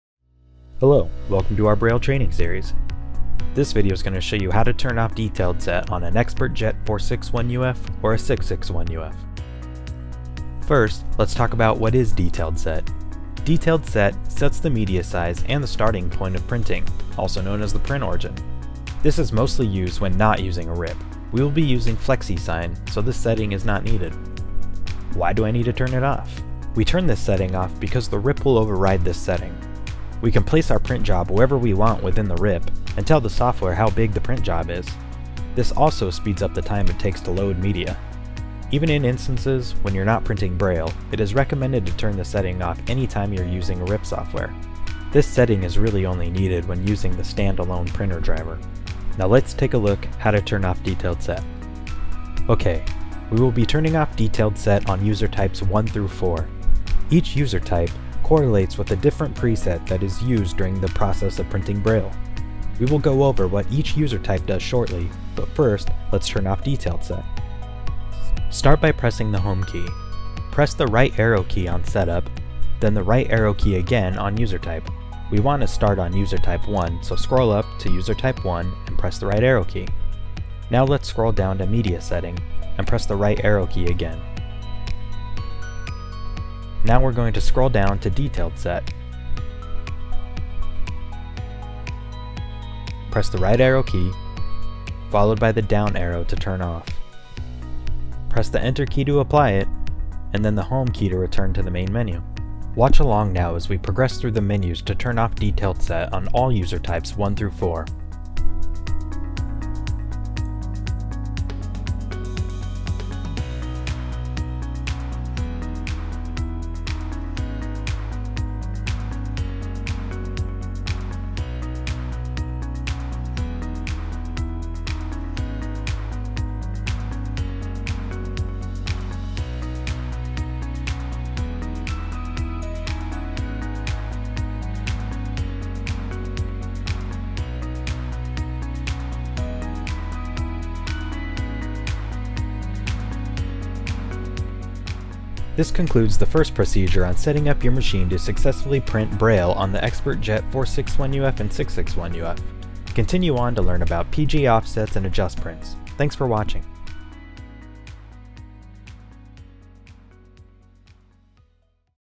How to turn off Detailed Set on Mutoh XpertJet 461UF/661UF
Consider this the first set when setting up your printer to bring Braille.  This video shows you how to turn off "Detailed Set" on your Mutoh XpertJet 461UF/661UF.